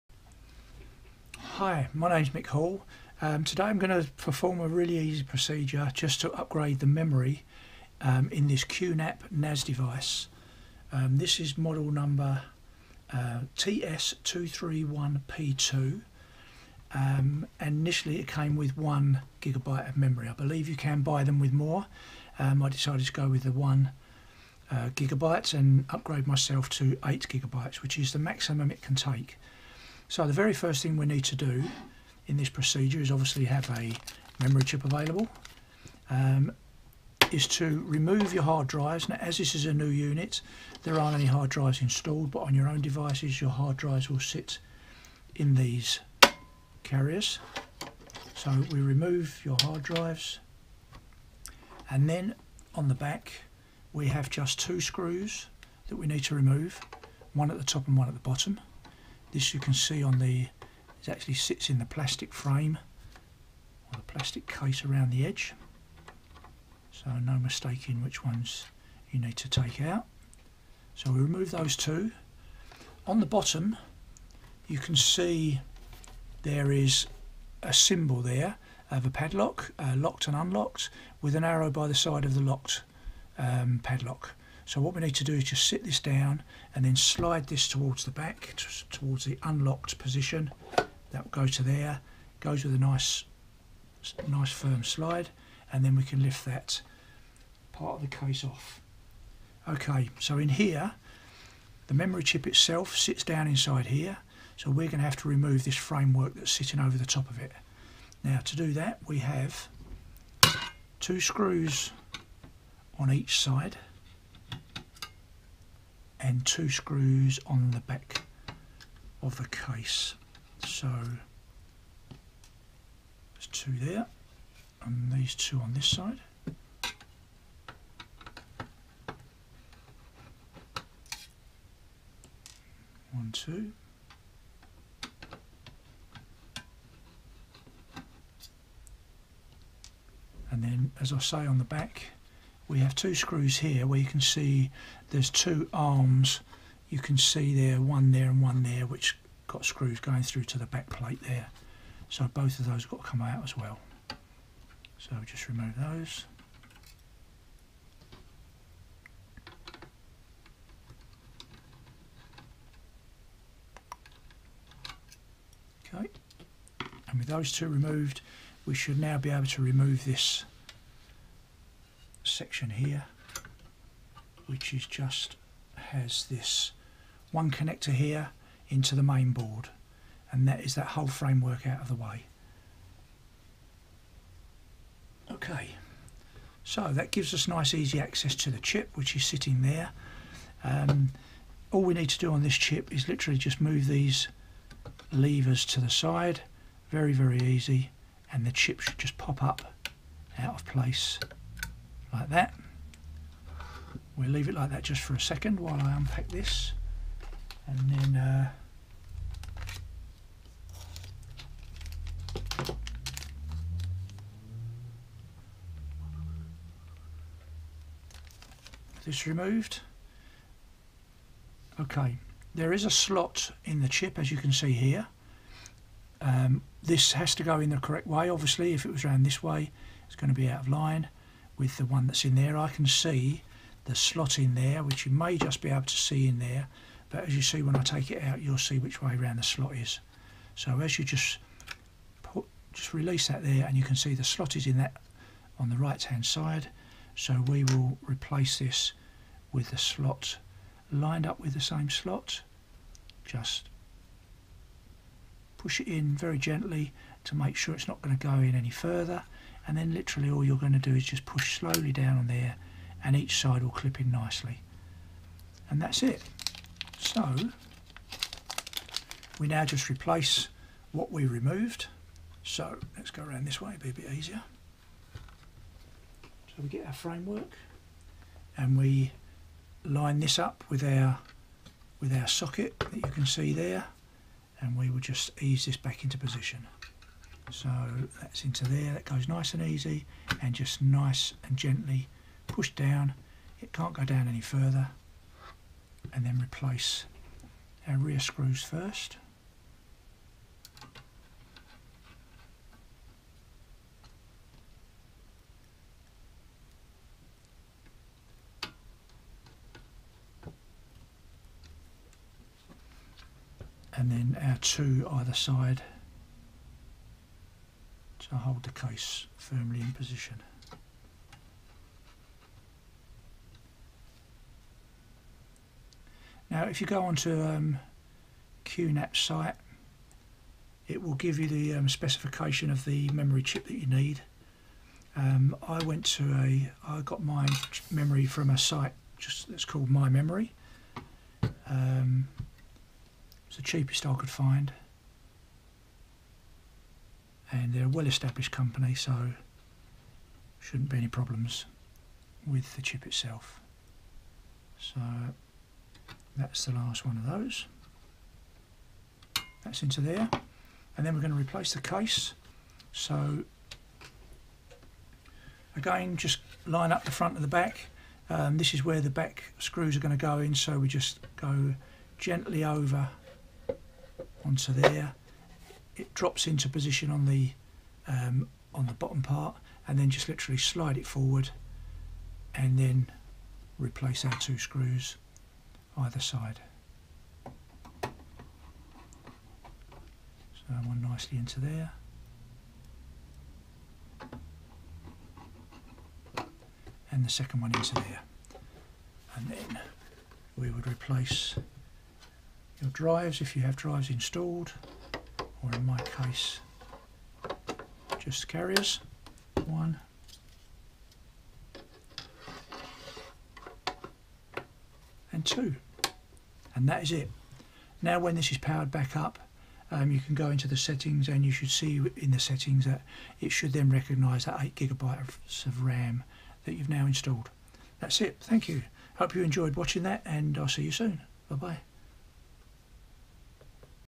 Qnap TS-231P2
Upgrading memory on Qnap TS-231P2